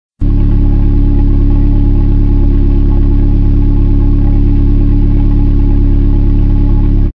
プレオのマフラーが腐ったのでヴィヴィオのマフラーを拾ってきました
- VIVIO muffler(silencer) on PLEO -
色や出口（カッター部）から見て、たぶん純正ノーマル同等品（補修用安価社外品）。

ヴィヴィオ用マフラーは（プレオ用に比べて）数ミリ短く、少し前に引っ張られる感じでタイコ位置が上がってしまうので（バンパーに出口部分が当たる）、吊り下げ位置を変えられる強化マフラーリングで下げました。